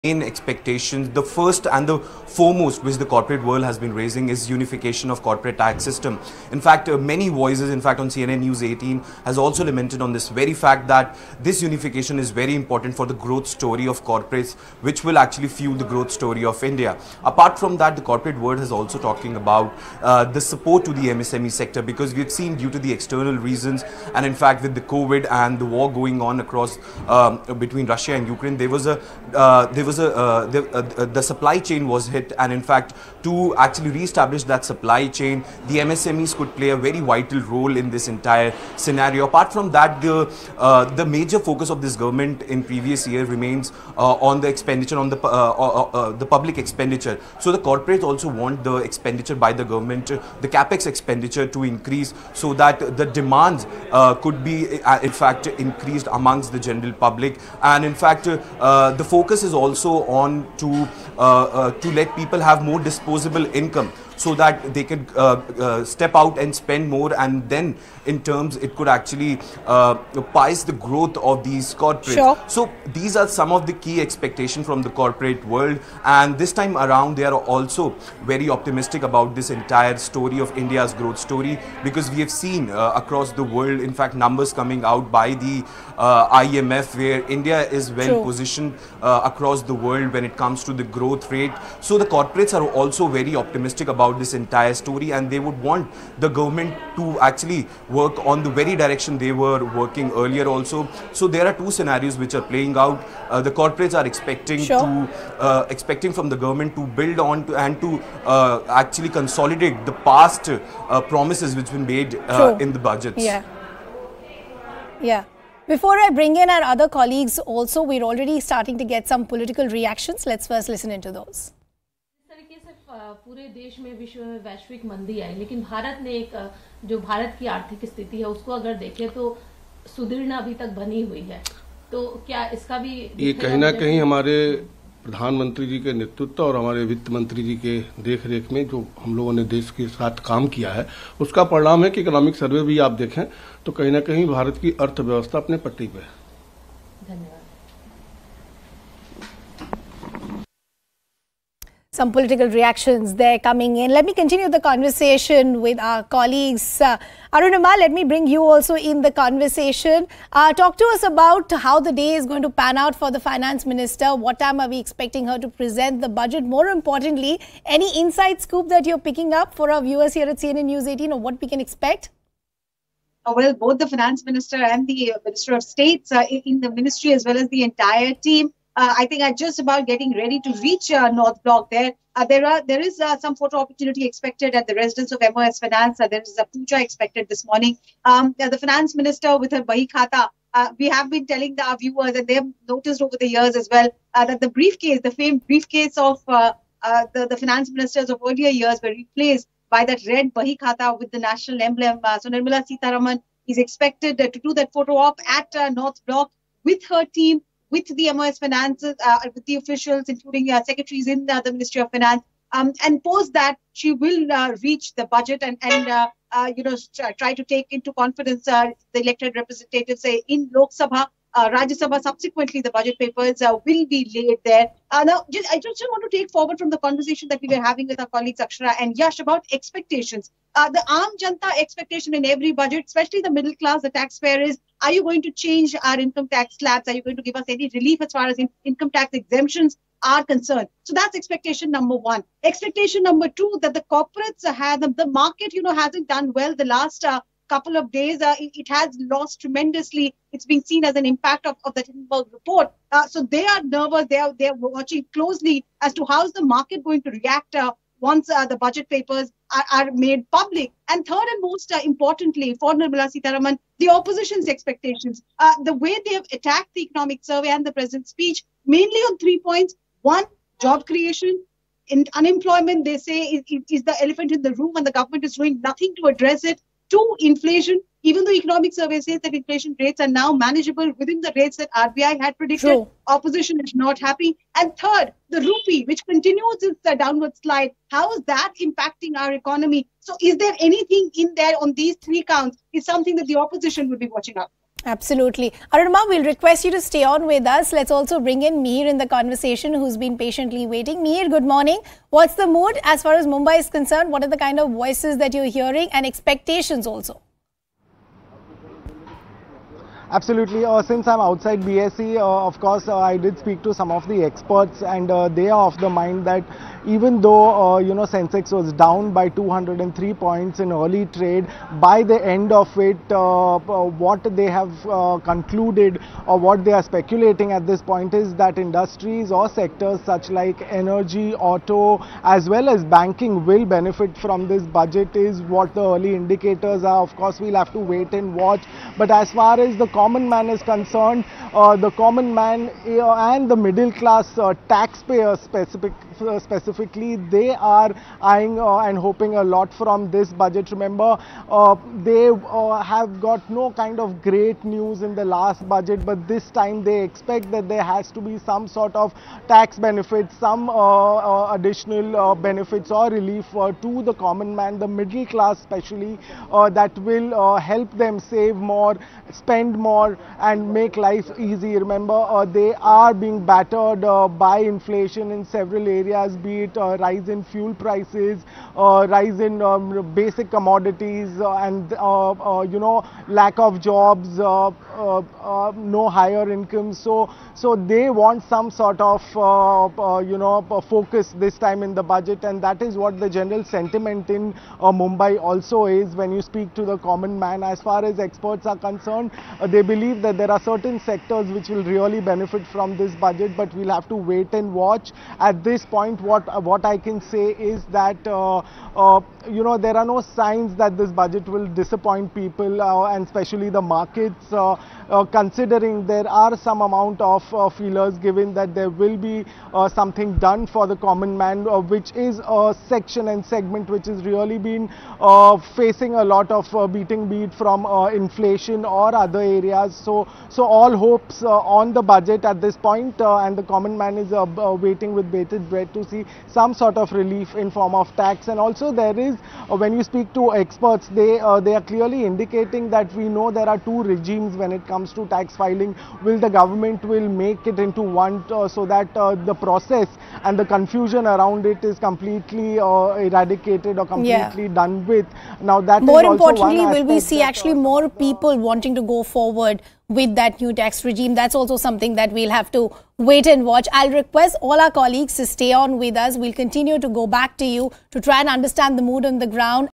Budget 2023 | Why A Decision On Cutting Taxes Will Not Be Easy For FM Nirmala Sitharaman? | News18
Budget2023 | Why a decision on cutting taxes will not be easy for FM Nirmala Sitharaman